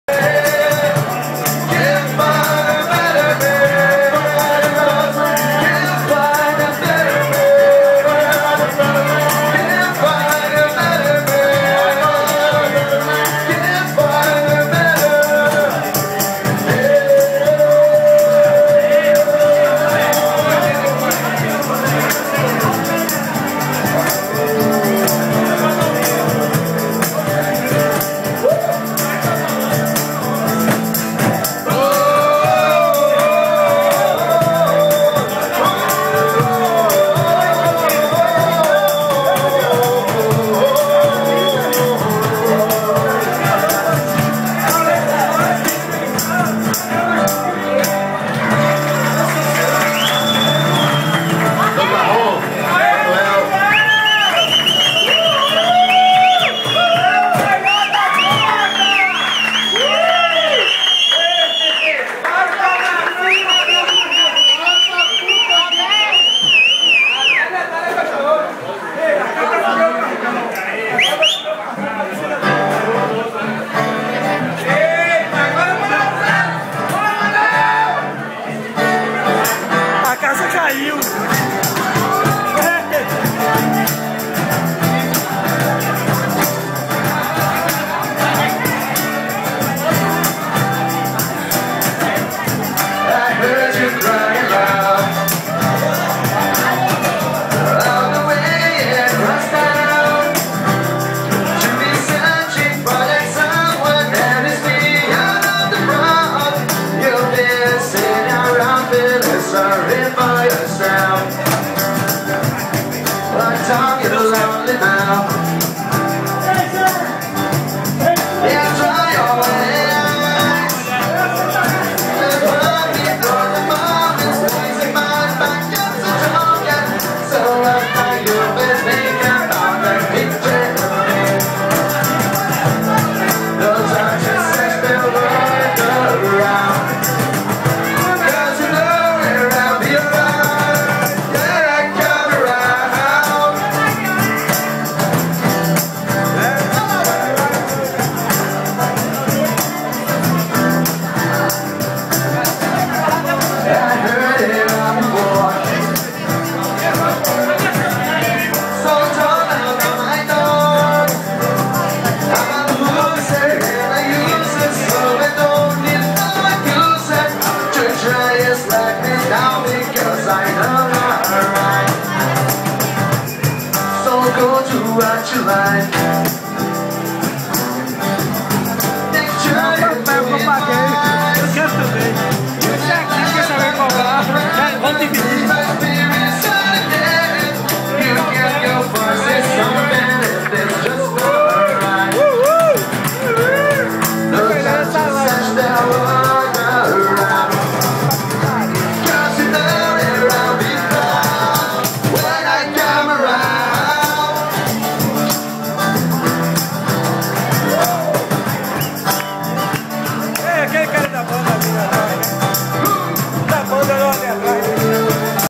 Aniversário de Manel
Aniversário de Manel no bar Fiteiro com banda LAS.